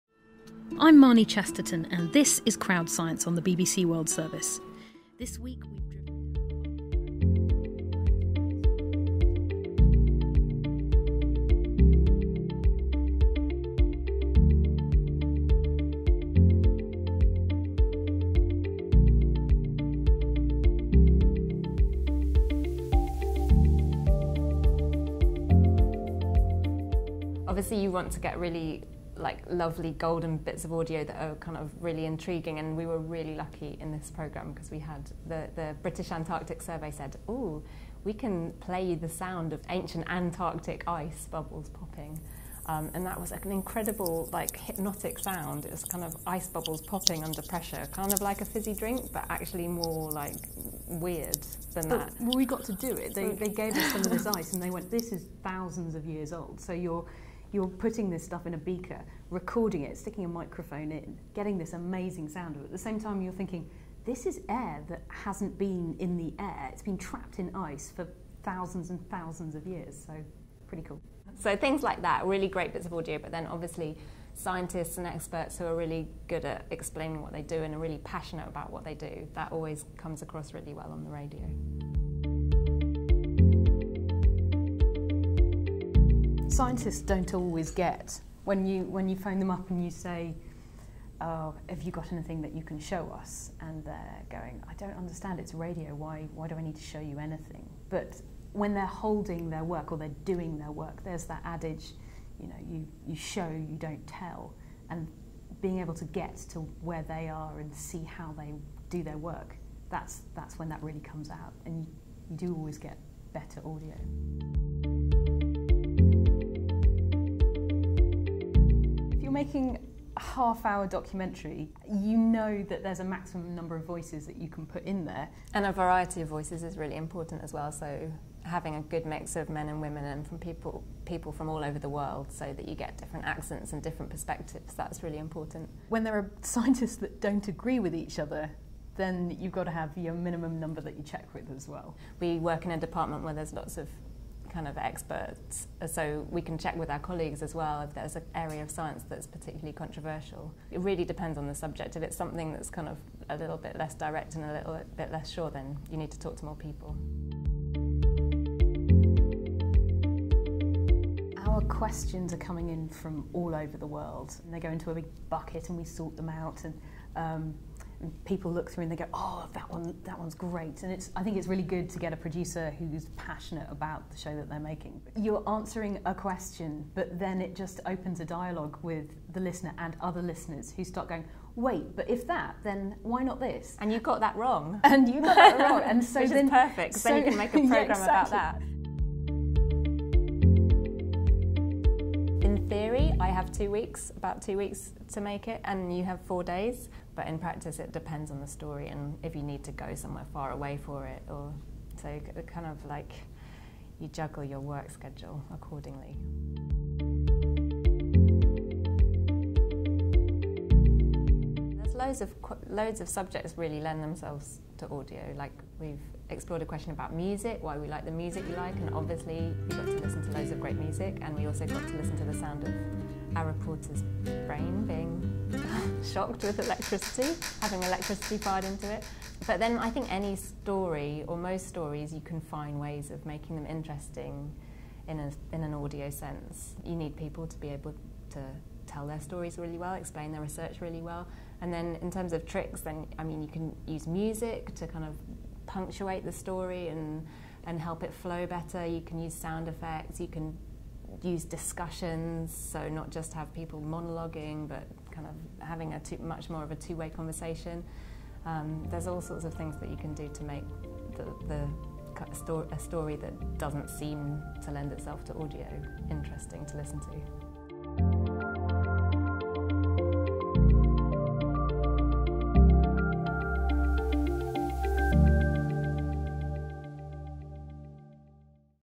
Ancient Bubble Pops: Mining stories for audio gold with Cathy Edwards and Marnie Chesterton
Cathy Edwards and Marnie Chesterton of BBC’s CrowdScience discuss reporting science for the radio, including working with scientists and how to use audio to tell a story. Their story on CO2 levels won the 2018 Gold Award in Audio.

for more information on the AAAS Kavli Science Journalism Awards.